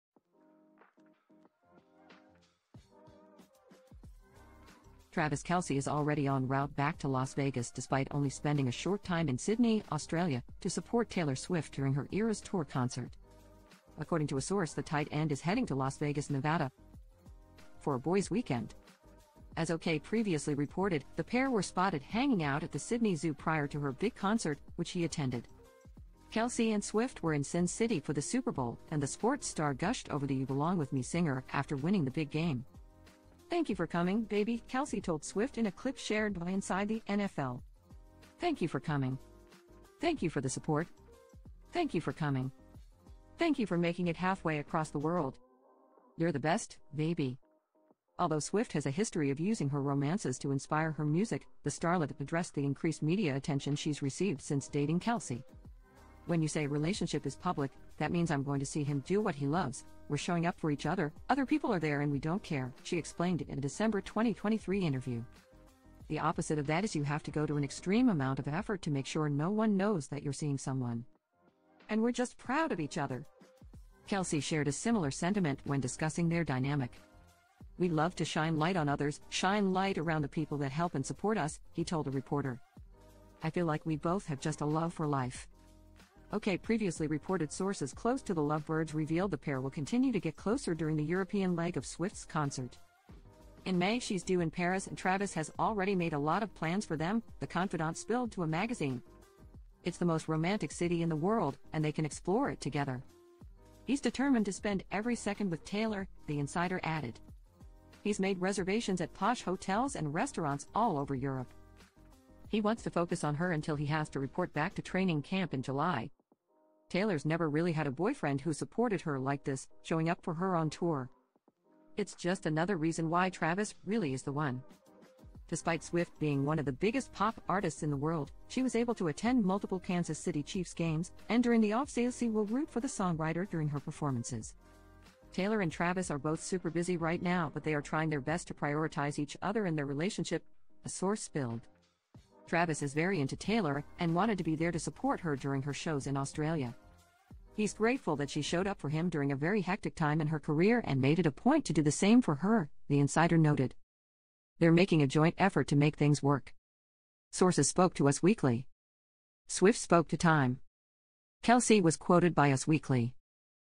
Travis Kelce Returns to Las Vegas for a 'Boys' Weekend' After Whirlwind Getaway to Visit Girlfriend
Travis Kelce is already en route back to Las Vegas despite only spending a short time in Sydney, Australia, to support Taylor Swift during her Eras tour concert.